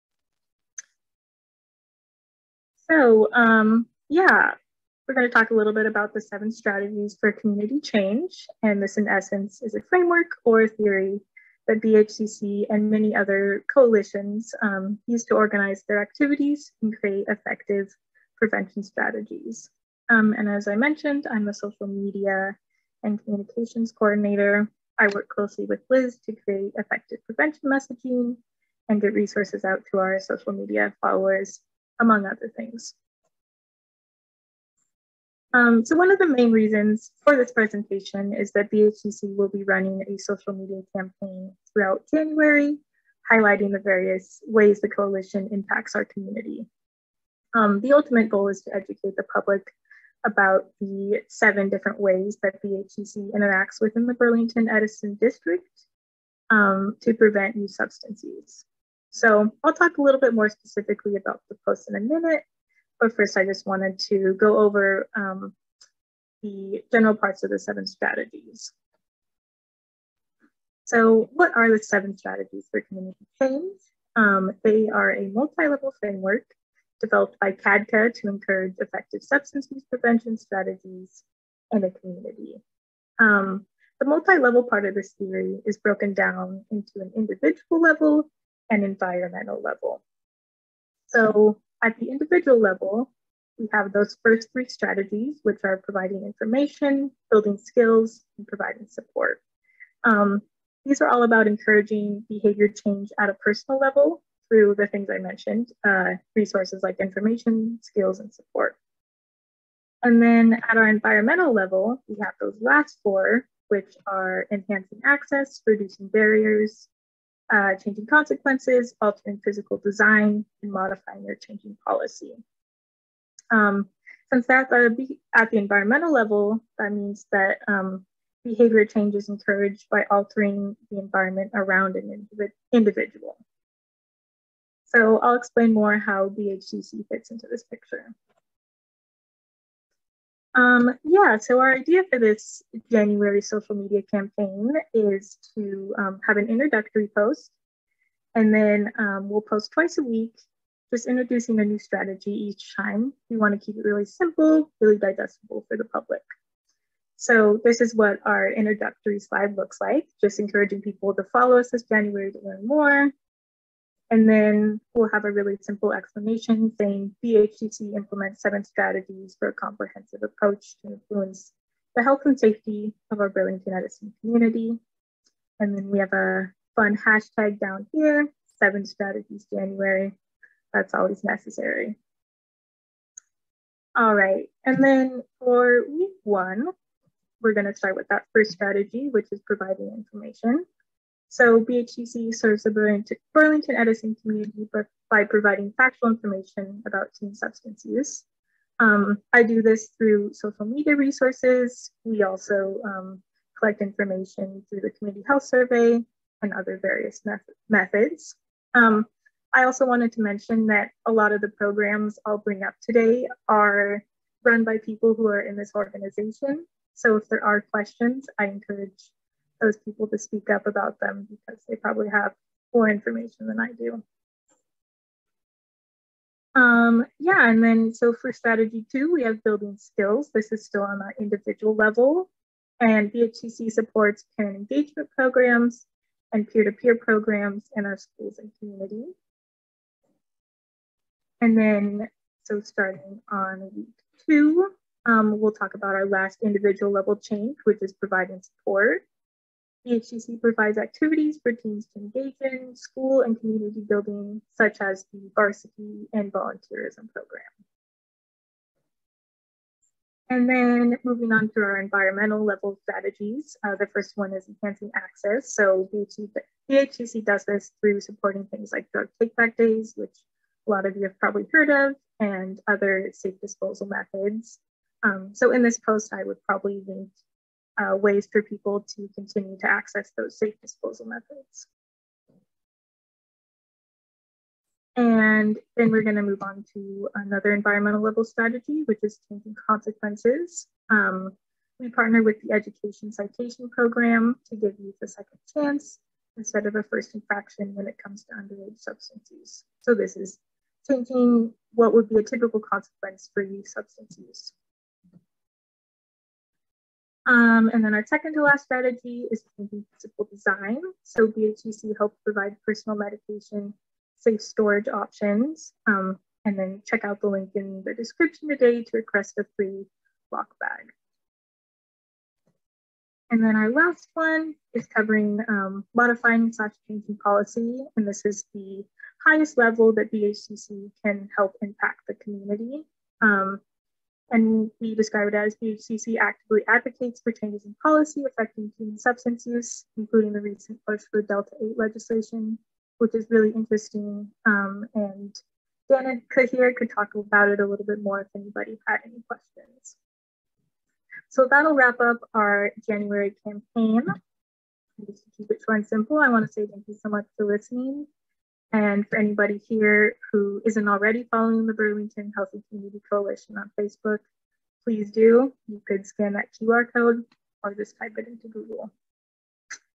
Burlington Healthy Community Coalition - 7 Strategies for Community Change
A coalition member speaks about the seven strategies for community change at a recent Burlington Healthy Community Coalition (BHCC) meeting.